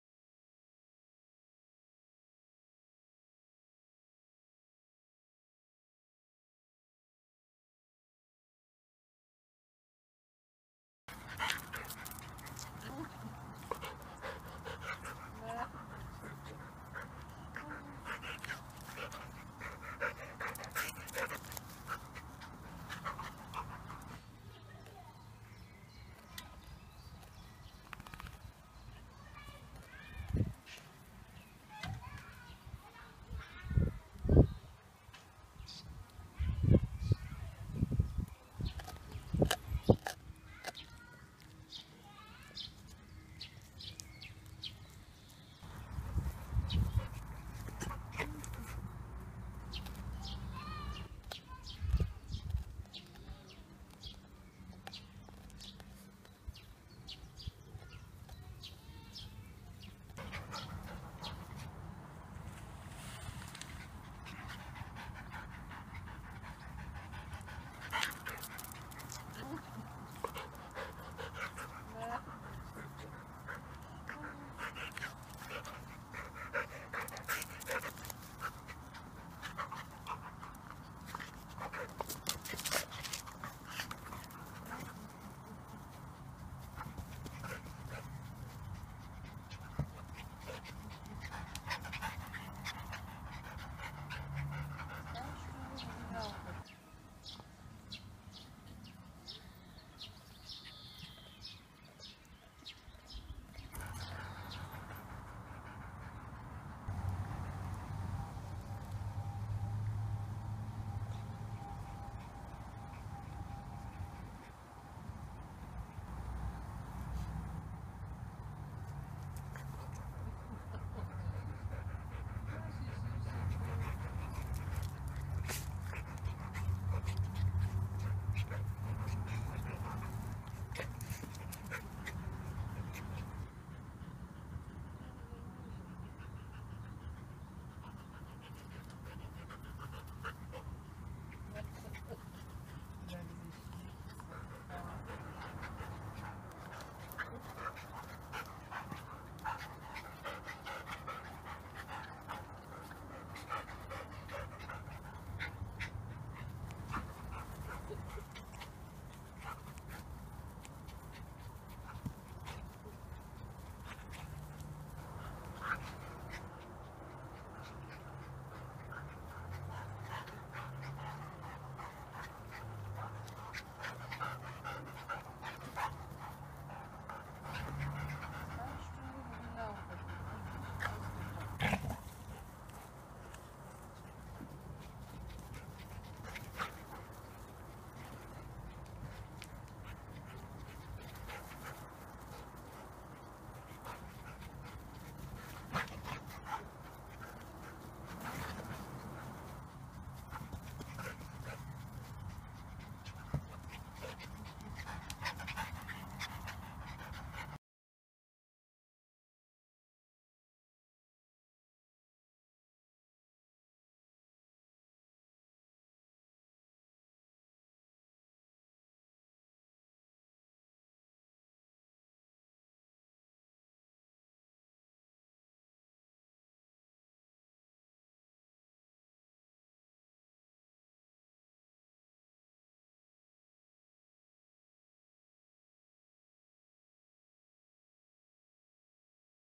Frida und Limes im Sommer 2015 (ohne Musik)
Norfolk-Terrier-Hünding "Frida" und "Limes", der Labradoodle, im Sommer 2015. Sie spielen, toben und laufen um die Wette (leider aus urheberrechtlichen Gründen ohne Musik)